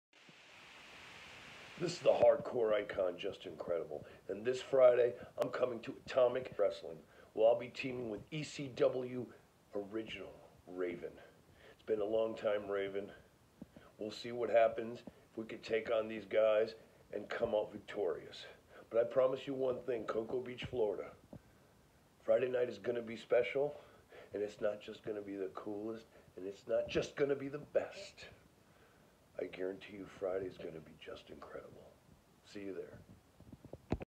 ECW legend JUSTIN CREDIBLE has a message for his match this Friday at ARW
This Friday February 21st at the Space Coast Convention Center in Cocoa Florida - ECW legends RAVEN & JUSTIN CREDIBLE will team together in a rare tag team match to take on BROTHERS IN ARMS for the Atomic Tag Team Championships. JUSTIN CREDIBLE has a special message for Brothers In Arms and the ARW Nation.